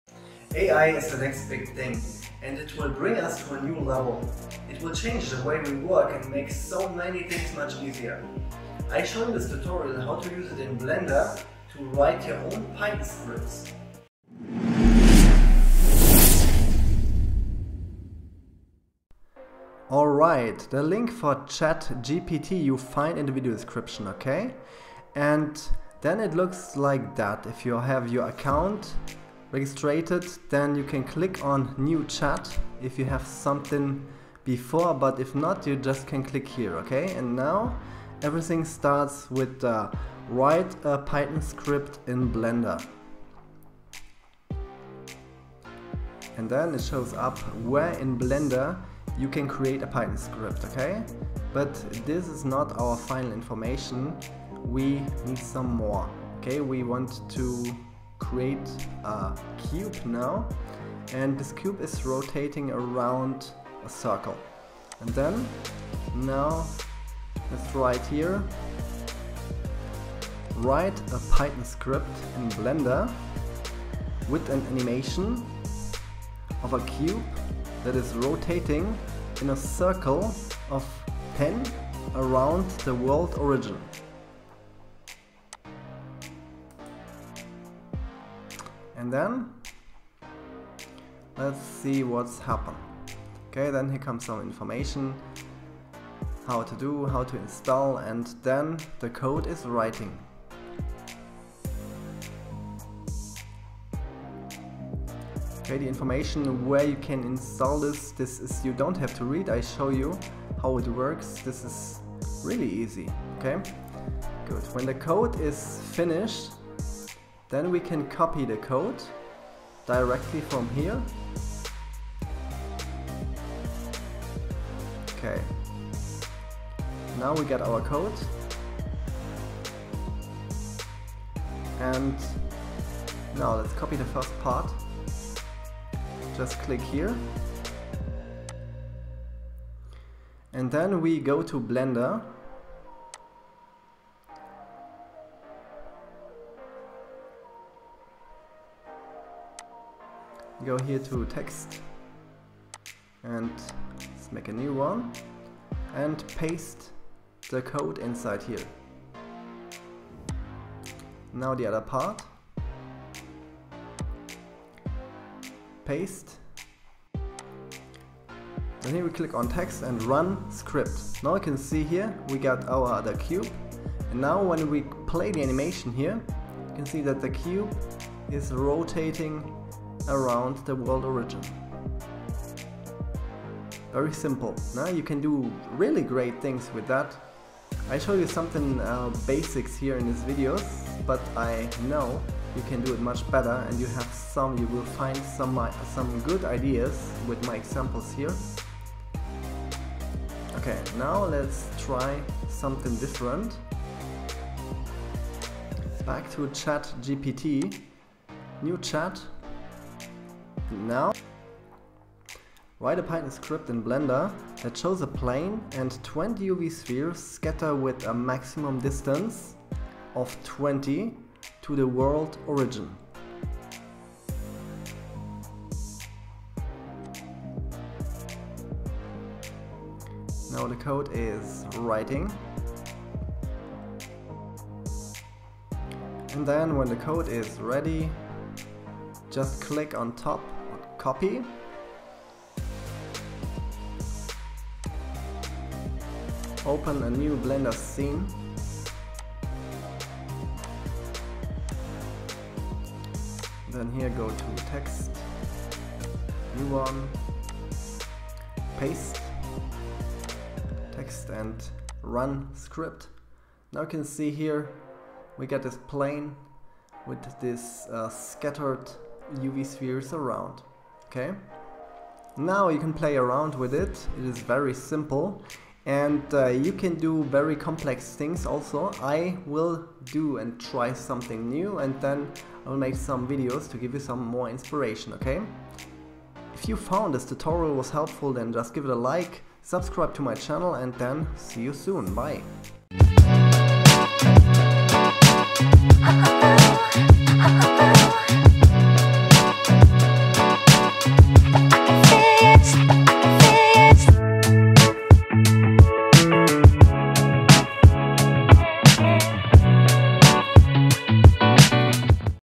How to create AI python scripts for blender with chat GPT
In this tutorial i show you how to create any kind of AI python scripts for blender with using Chat GPT!

Create your own video games without coding